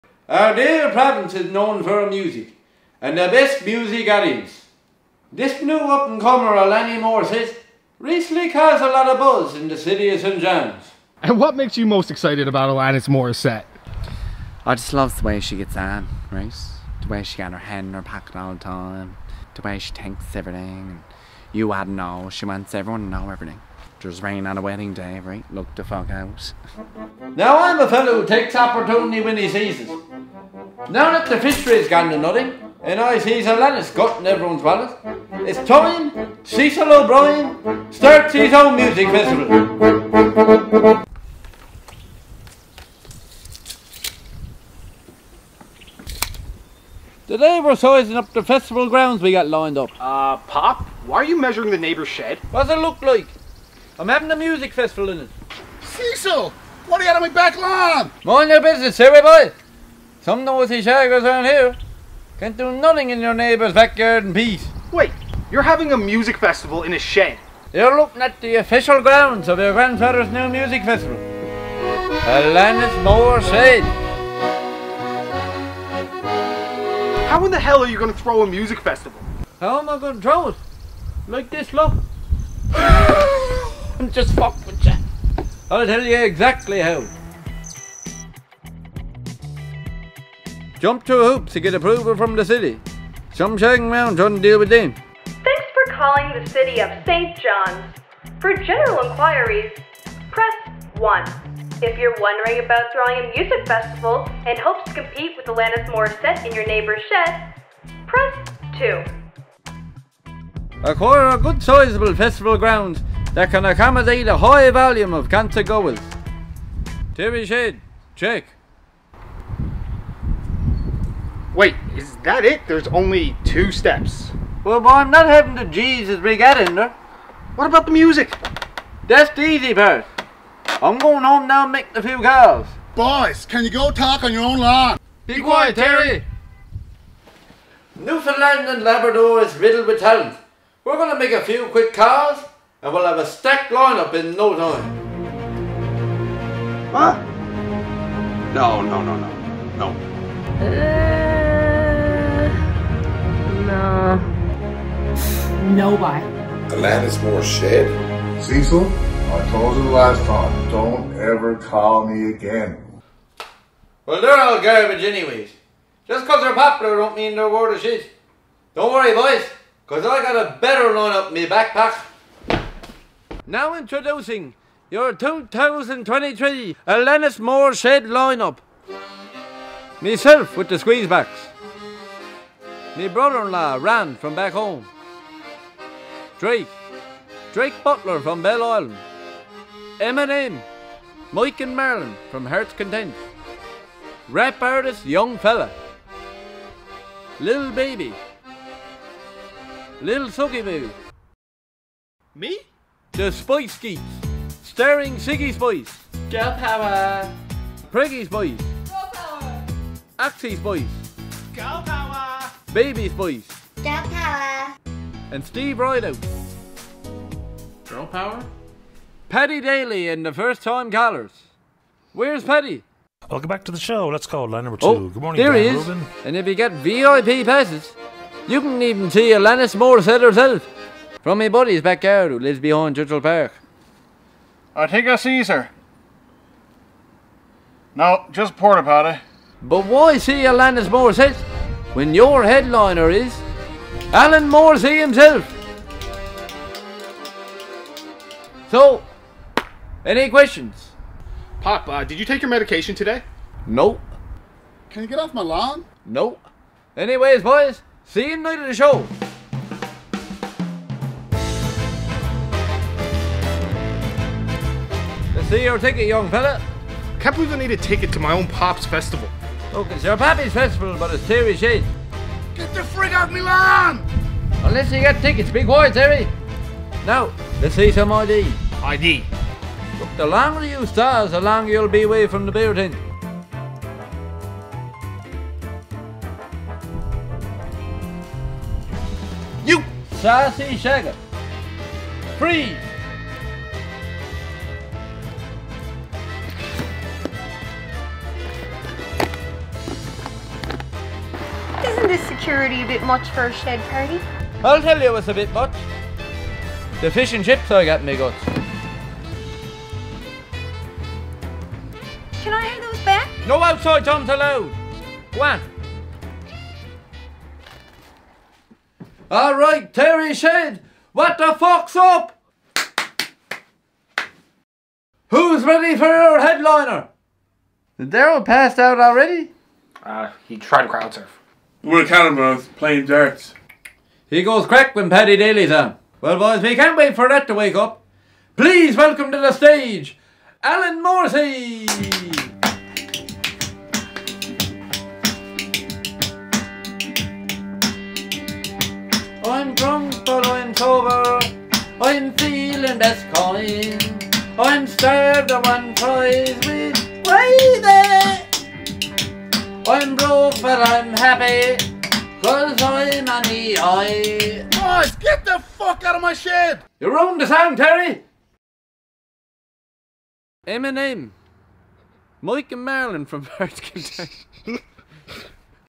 Cecil vs Alanis Morissette
Cecil makes his own music festival to try and get Alanis Morissette money